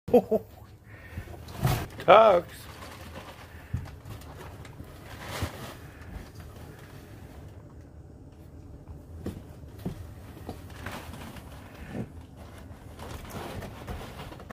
Black Cat Whack-A-Mole Saint Bernard Puppy
Max our St Bernard playing whack a mole with our cat Tux.